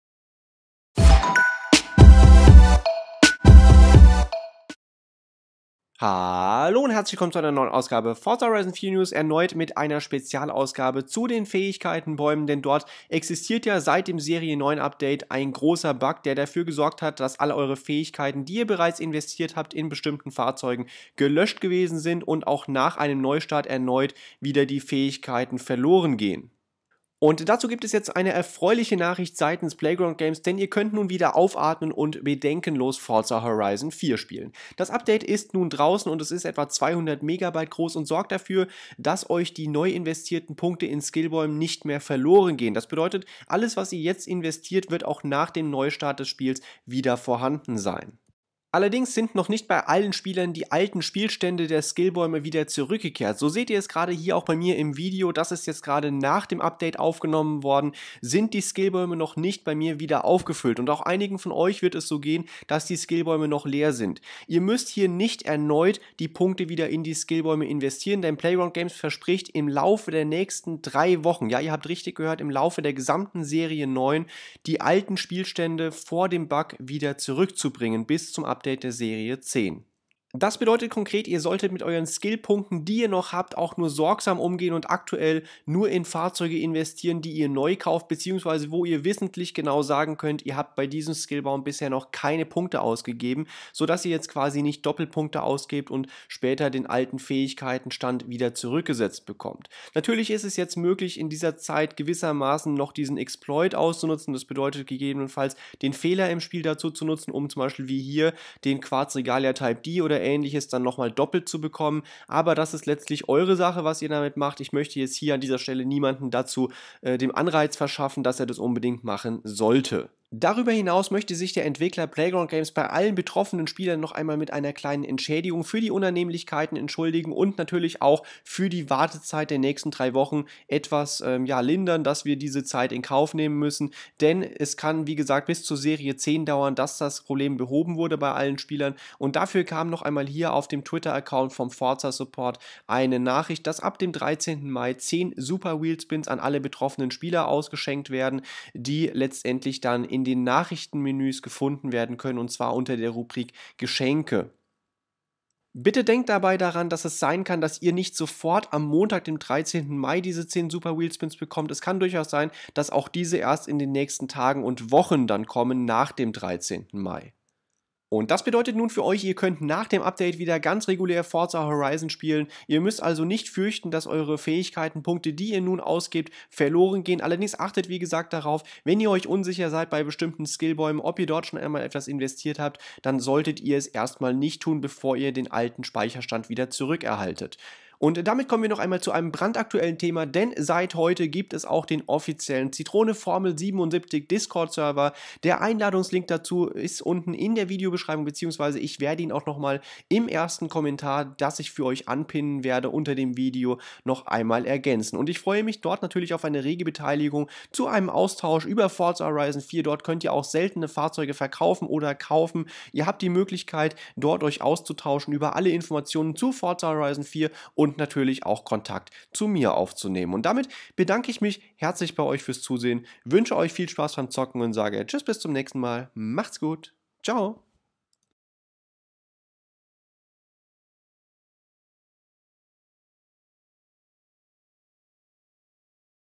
Forza Horizon 4 News - Der Fix ist da plus Entschädigung für betroffene Spieler!
News Ausgabe vom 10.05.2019 mit dem Patch für den Fähigkeitenbaum-Bug seit Serie 9.